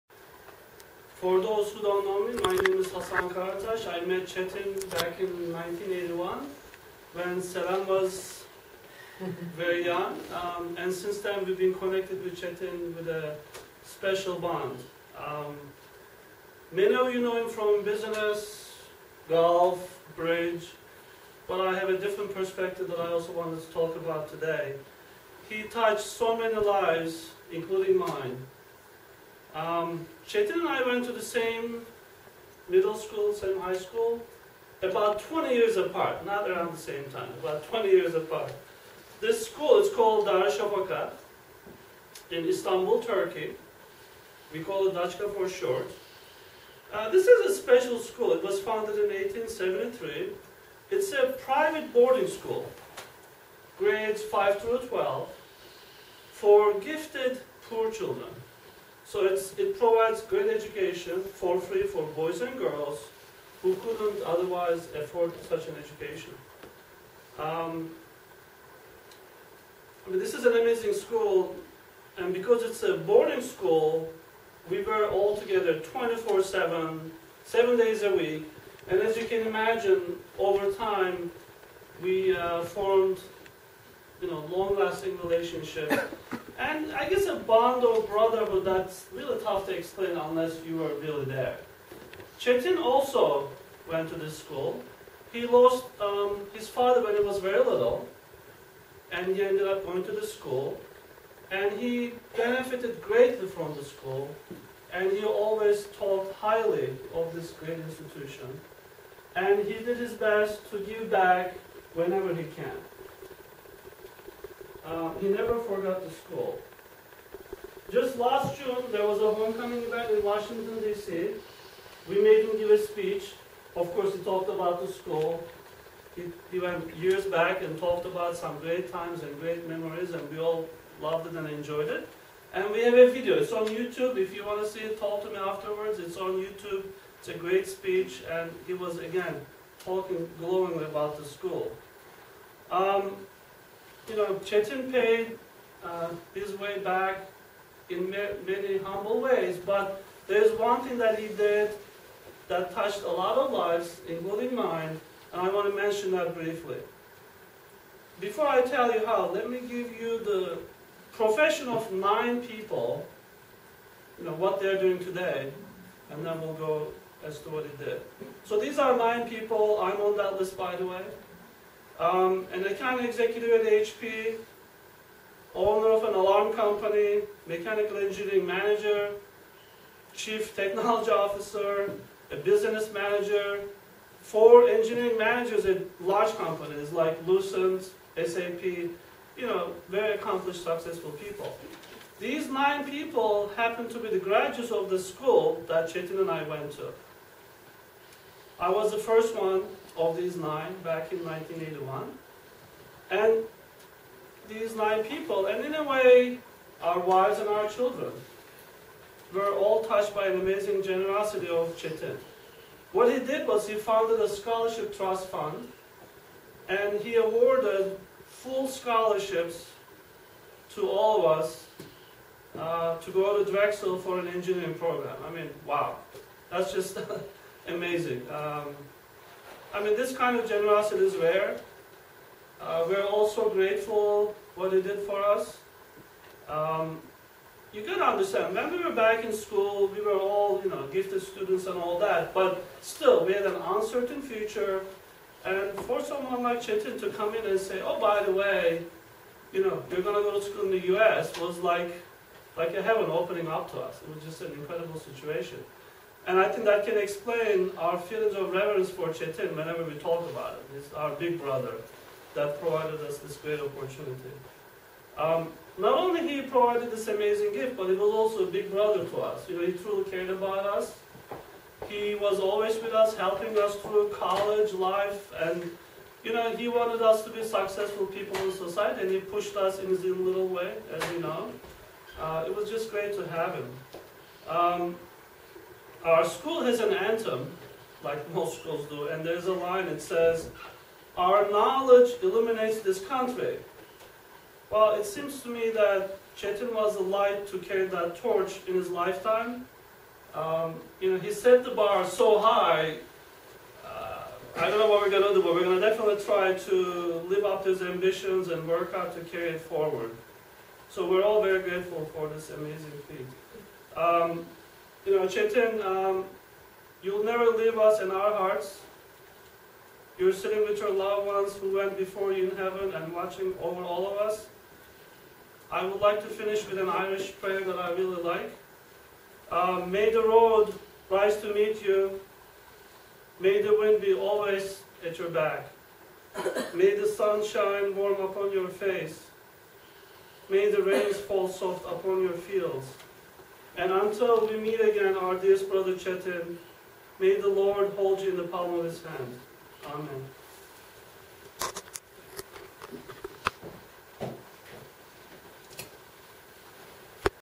Cetin A. Okcuoglu, Darussafaka Class of 1960 Memorial Speech by Hasan DS'81
Hasan Karatas DS'81 speaks at
Cetin A. Okcuoglu DS'60 Agabey's Memorial
in Moorestown, NJ
on April 6, 2013
Orange Nikon Cam DSCN1152